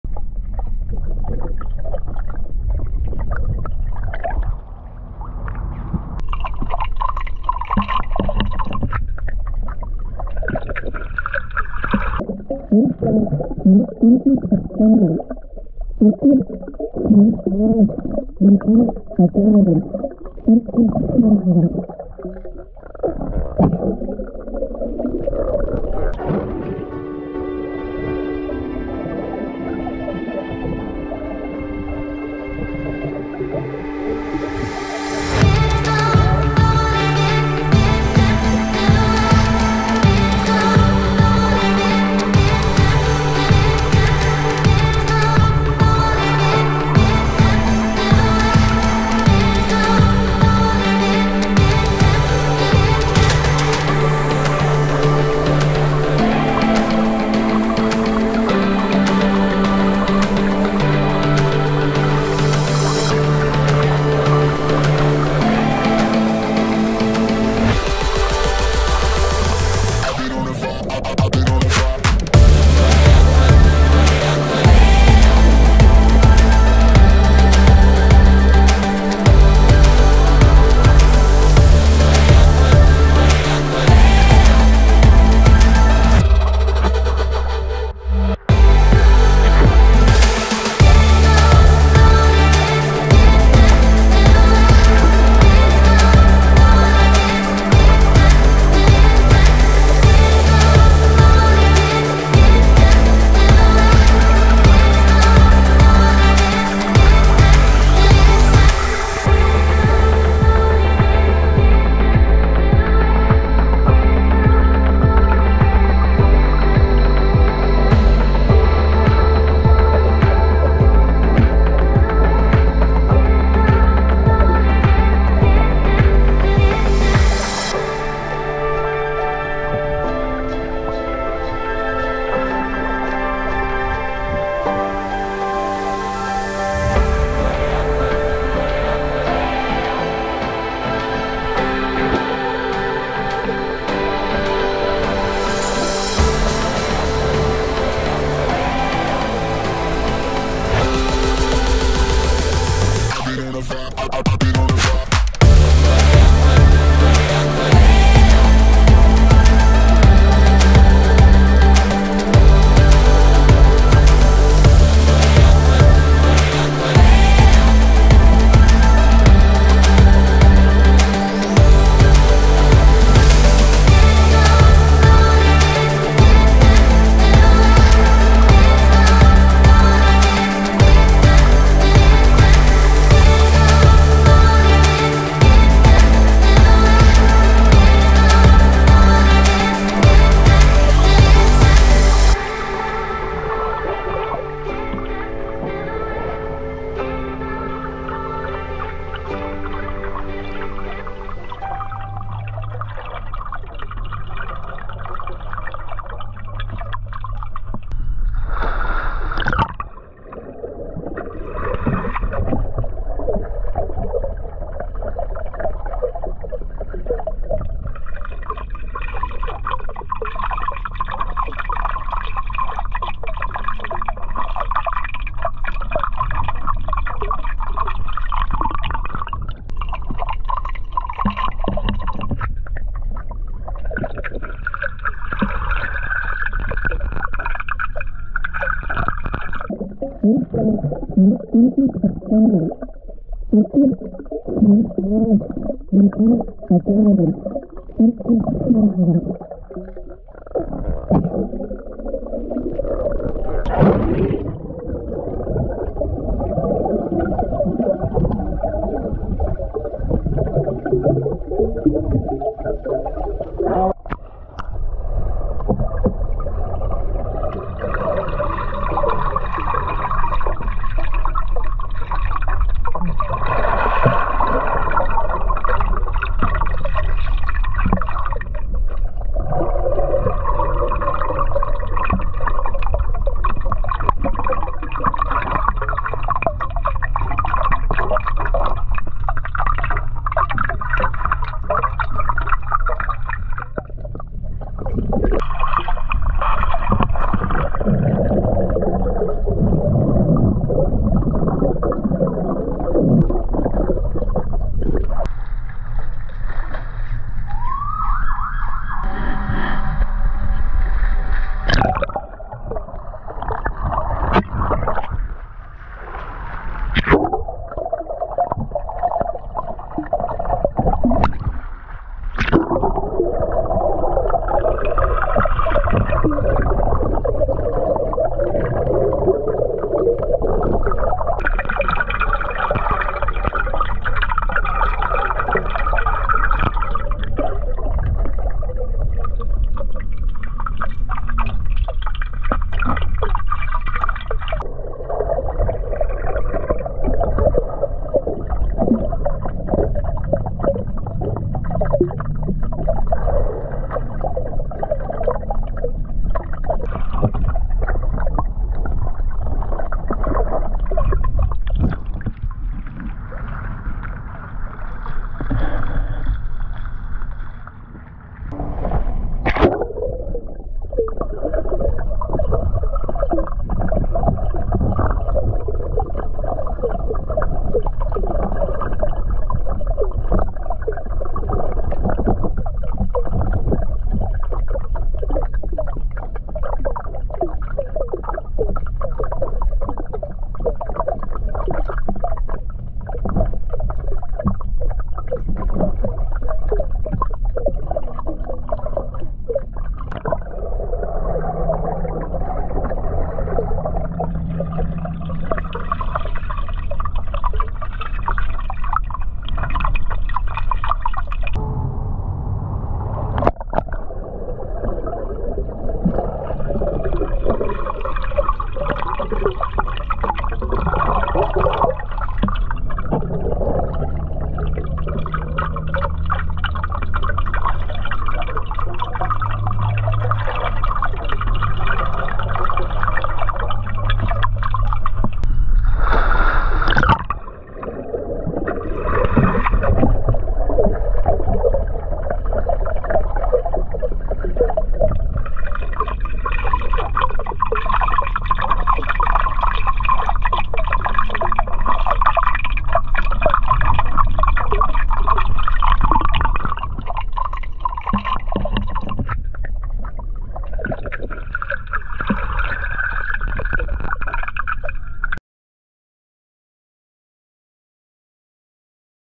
Underwater Summer Swimming In Winter Clothes - Boots | Fleece Jacket and Scarf
Hi Guys!
Swimming underwater wearing winter clothes
In this video I celebrate a new milestone on YouTube.
All I can say is THANK YOU!!!
Love YOu ❤️
SheViking
(Anna)